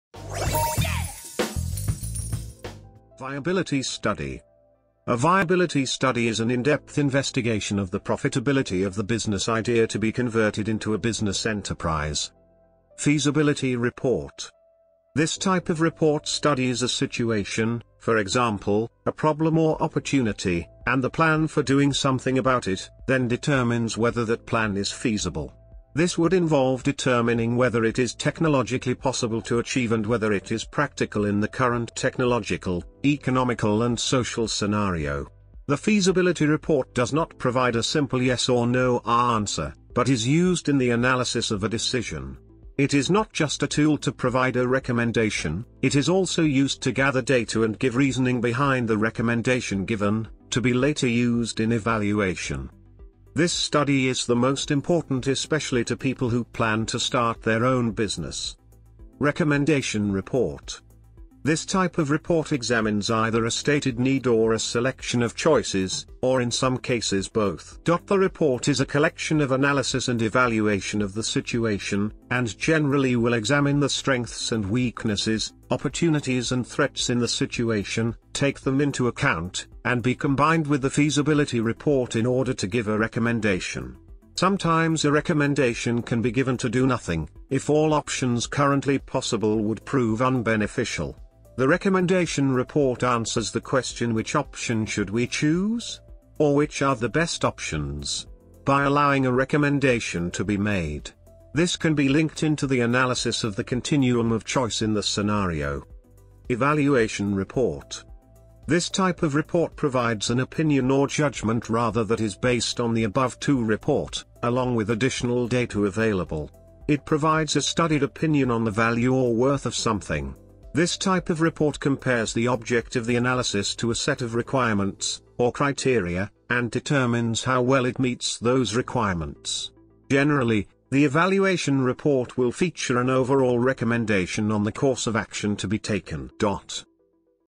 Viability study 📈💲 BUSINESS TERMS 💲📉
In this video series we're covering everything about BUSINESS TERMS.

A Viability study is an in depth investigation of the profitability of the business idea to be converted into a business enterprise.